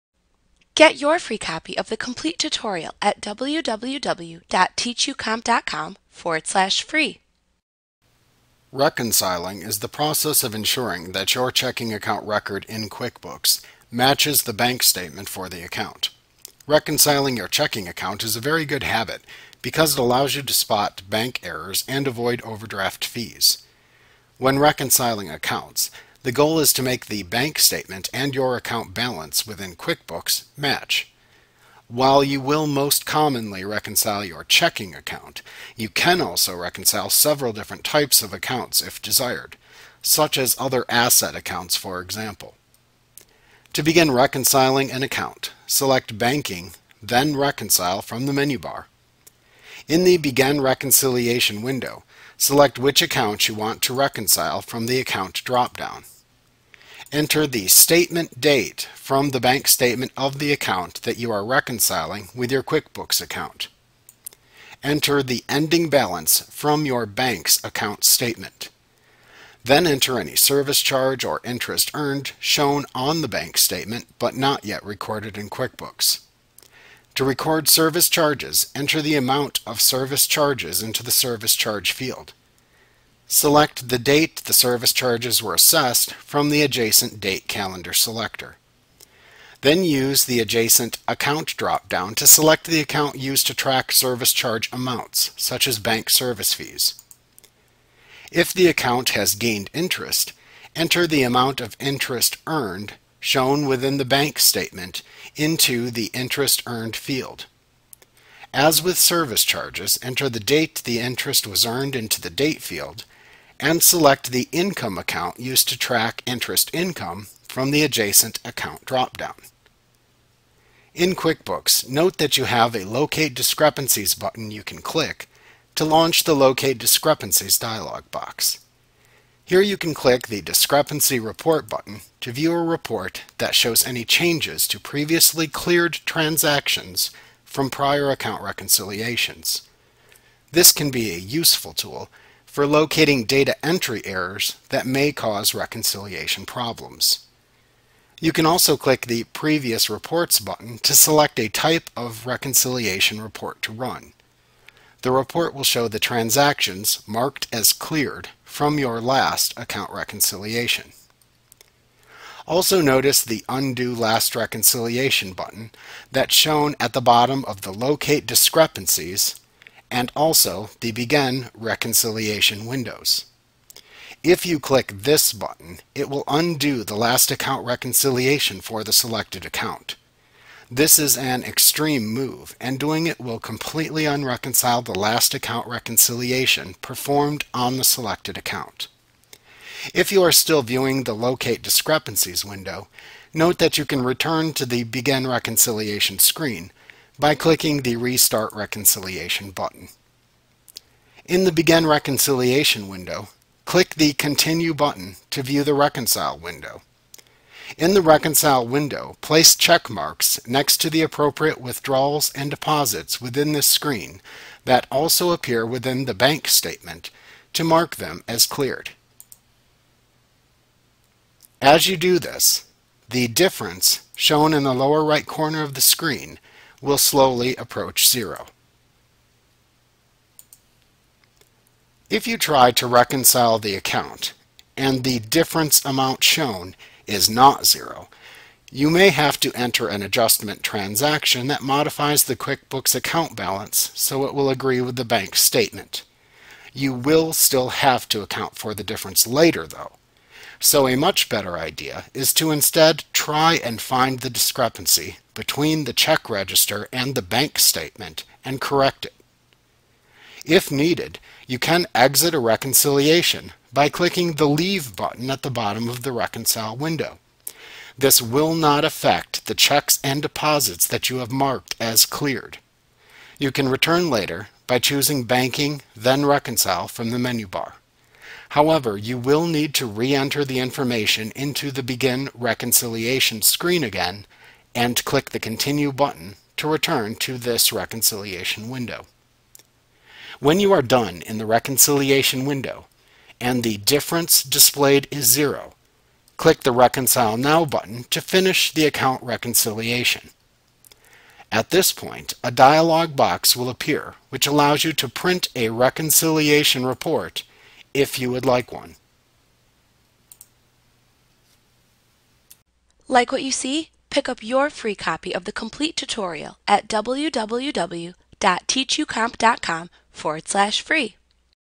QuickBooks Pro 2015 Tutorial Reconciling Accounts Intuit Training
Learn about Reconciling Accounts in QuickBooks Pro 2015 A clip from Mastering QuickBooks Made Easy. - the most comprehensive QuickBooks tutorial available. Visit us today!